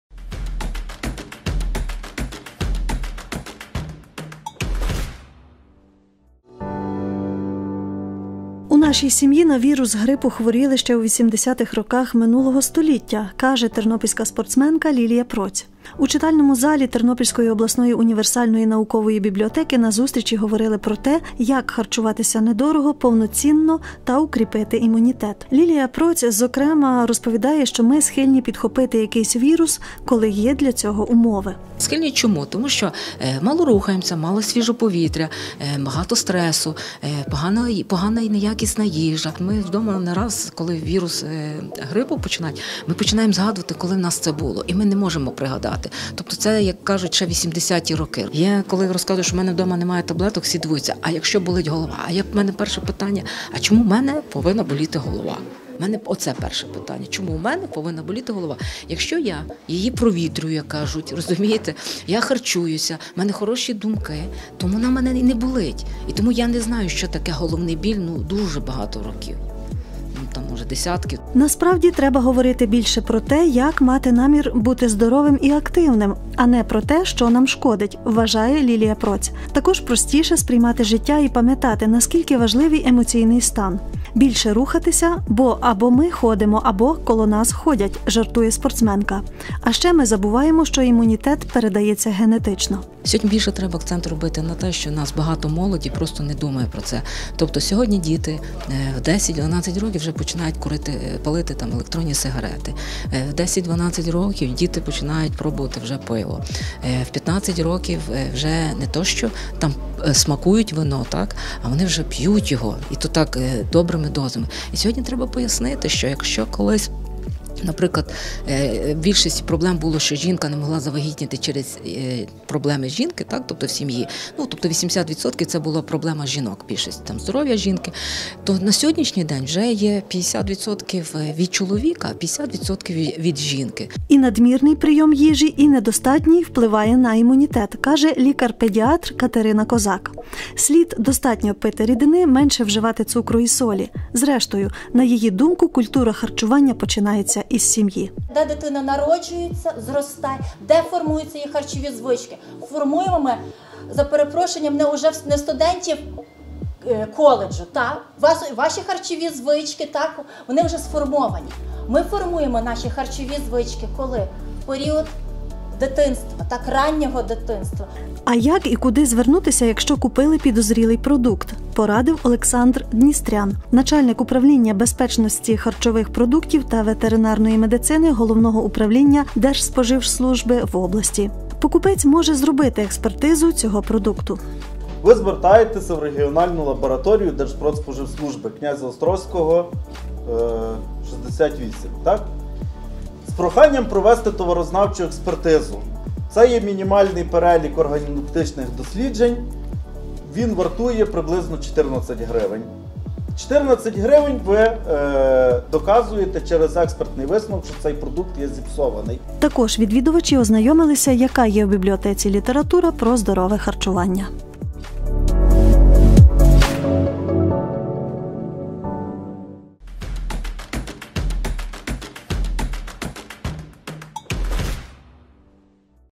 Сюжет- Здорове харчування.
Здорове харчування має багато переваг, і є дуже важливим для кожної людини. Як збалансувати свій раціон і укріпити імунітет знає Наталя Лазука.